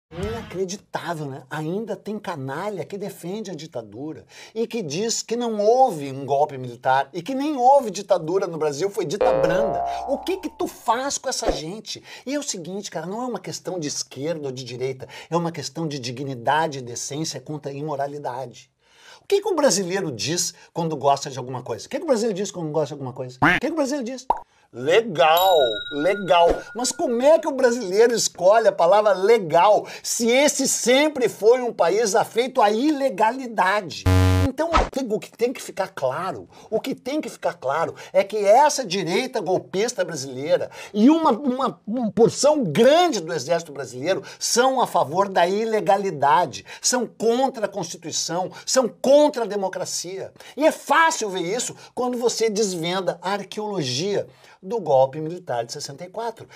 O DIA DO GOLPE - EDUARDO BUENO
Não há melhor dia que o 1º de abril para relembrar um dos momentos mais tolos da história do Brasil quando, em 1964, o povo insuflou os militares para saírem da caserna e, a cassetetes, baixarem o cacete enquanto meia dúzia de cadetes acabavam com a própria democracia - a nossa democracia.